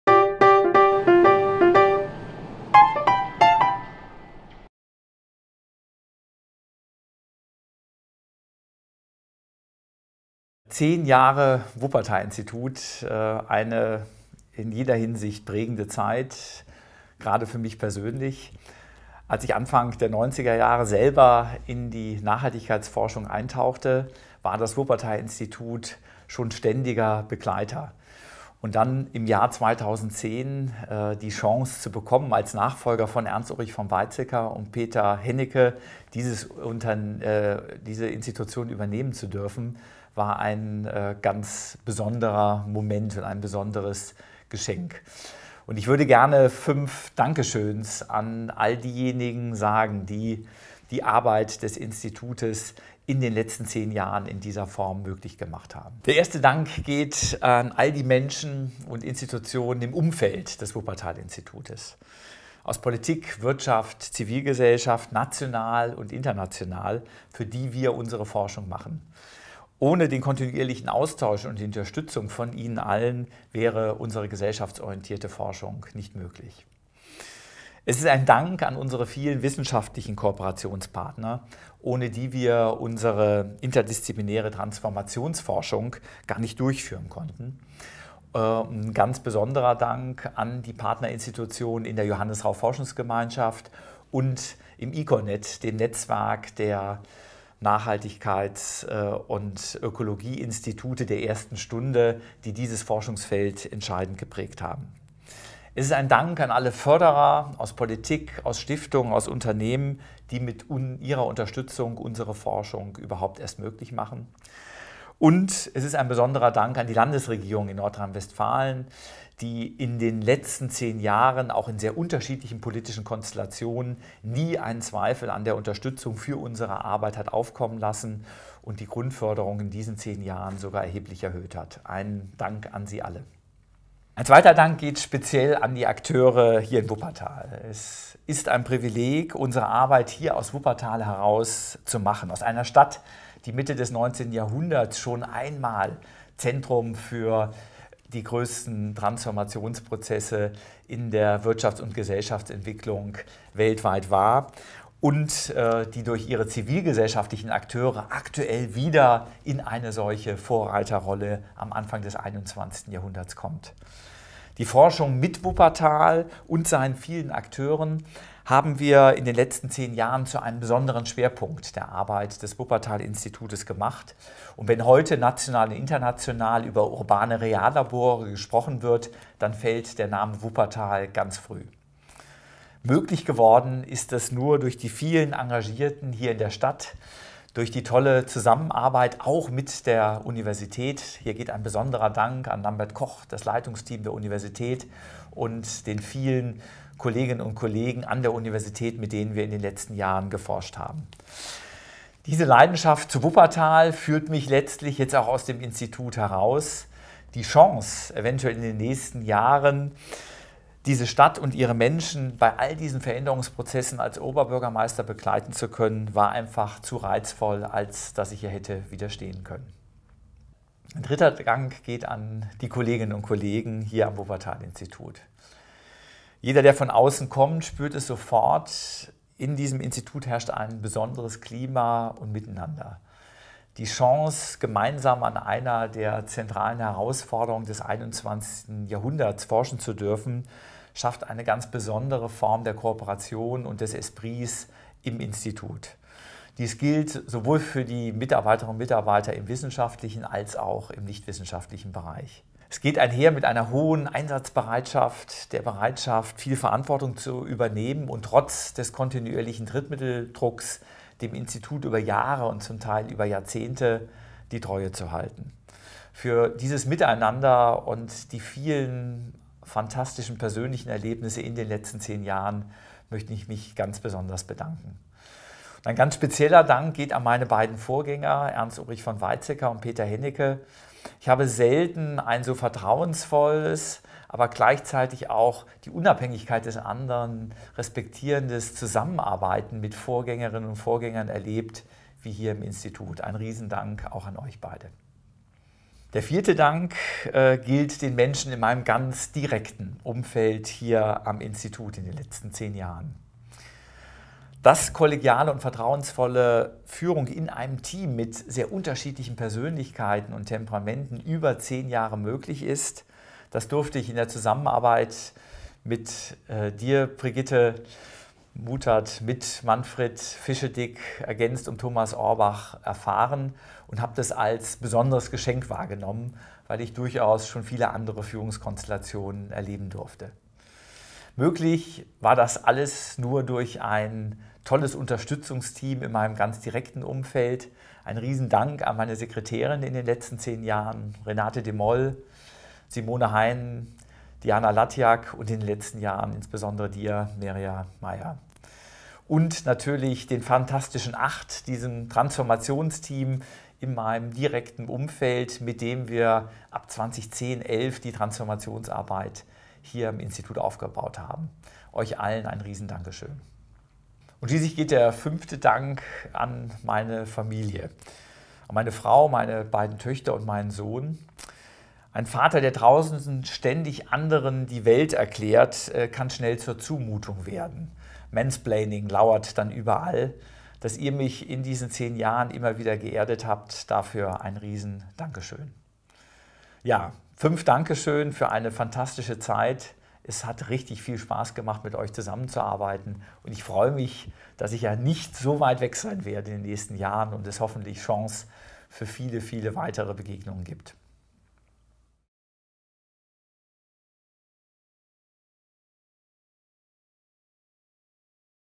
10 Jahre wissenschaftlicher Geschäftsführer des Wuppertal Instituts: Dankesrede von Uwe Schneidewind
Nach zehn Jahren verließ Prof. Dr. Uwe Schneidewind das Wuppertal Institut als Präsident und wissenschaftlicher Geschäftsführer am 30. April 2020. Dieses Video wurde anlässlich seiner digitalen Verabschiedung am 24. April 2020 aufgezeichnet. Schneidewind ließ das vergangene Jahrzehnt in seiner Ansprache Revue passieren und bedankte sich bei seinen Kolleginnen und Kollegen sowie Wegbegleitenden und Partnerinnen und Partnern.

Filmische Umsetzung: Treibsand Film Kim Münster, Sebastian Bergfeld